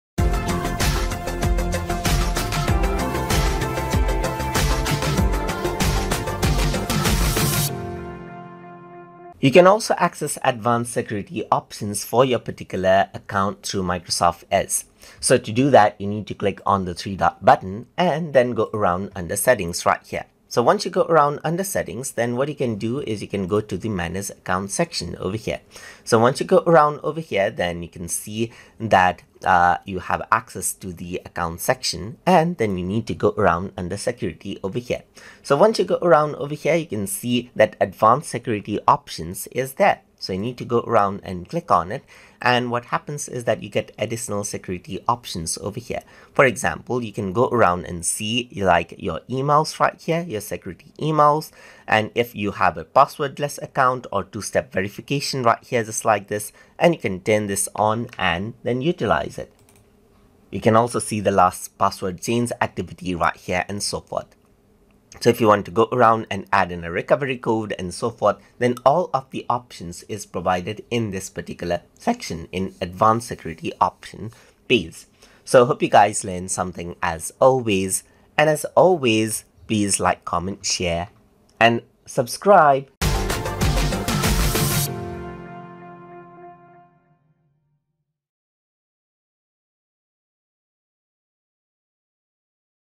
Microsoft Edge - Lesson 49 - Accessing Advanced Security Options
In this tutorial, we will be discussing about Accessing Advanced Security Options in Microsoft Edge

Learn the basics of using Microsoft Edge Browser including browse, manage, use of copilot and work with extensions.

This lesson has been made using Microsoft Edge for Windows but is applicable for App Version and Mac Version of Microsoft Edge Application as well.

If you like this video, here's our entire playlist of Microsoft Edge tutorials: